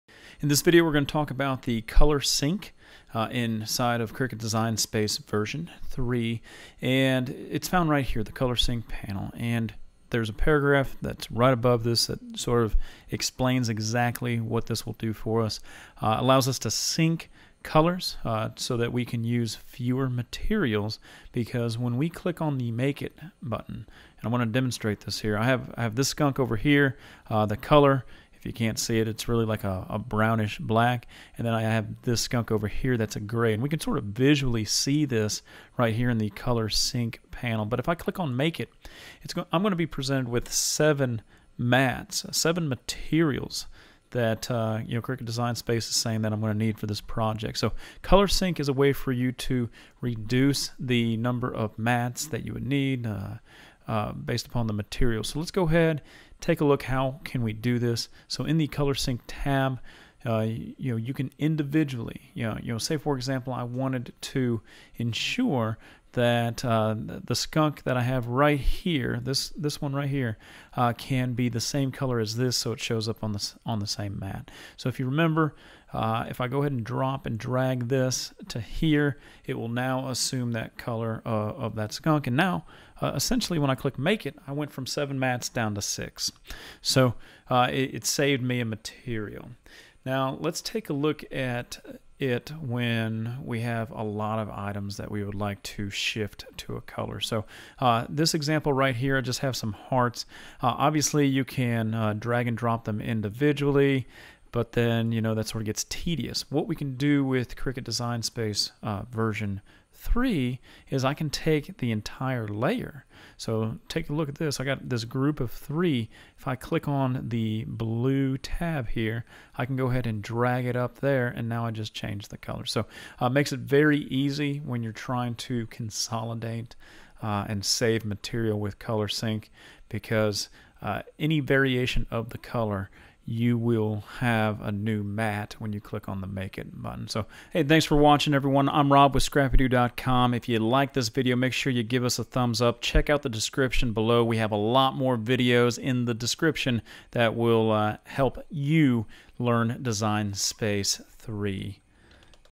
Design Space 3: Consolidate Colors with Color Sync
Use the Sync panel to consolidate colors of a project in order to reduce the number of different materials needed. You can sync layer colors by dragging an image layer and dropping it onto another layer in the color you want to match.